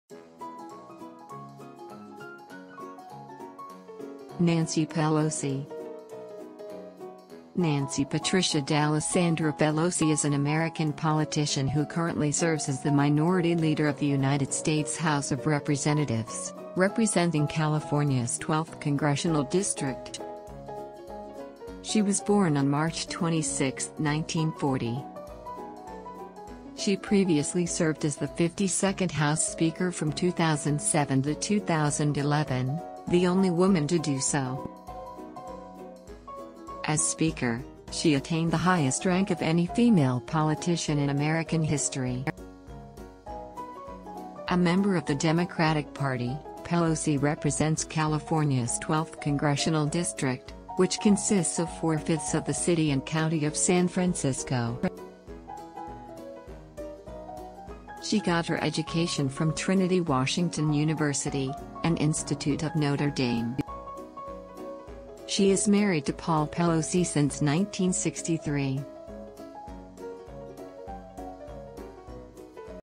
Nancy Pelosi: Everything you need to know...
Nancy Patricia D'Alesandro Pelosi is an American politician who currently serves as the Minority Leader of the United States House of Representatives, representing California's 12th congressional district. Watch this video to know Nancy Pelosi's story.